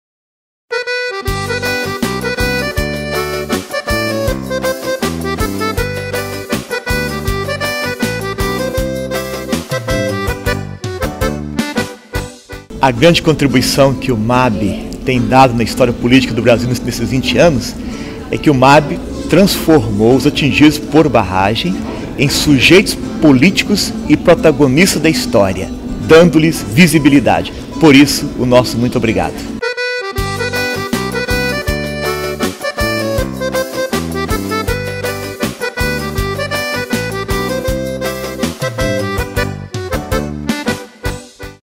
MAB 20 Anos - Pe. Nelito Dornelas (Assessor da CNBB)
Parabéns do assessor da CNBB, Pe. Nelito Dornelas, ao Movimento dos Atingidos por Barragens (MAB) pelos seus 20 anos de lutas e conquistas.